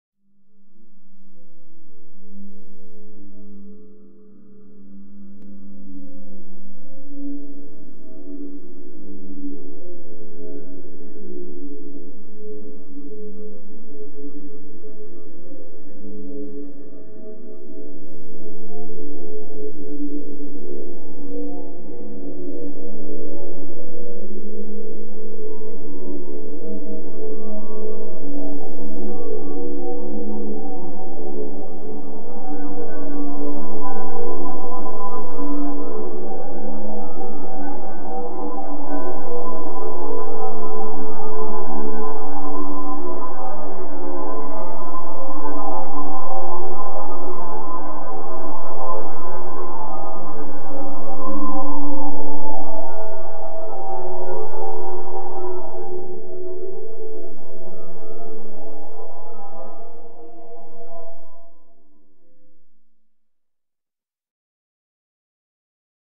Horror Alien Background | Scary Halloween Sounds
Scary Alien. Horror Atmosphere Sounds. Horror Background Ambience Sound Effect. Scary Horror Ambience. Great sound clip for video clips, games, commercials, apps. Free MP3 Download.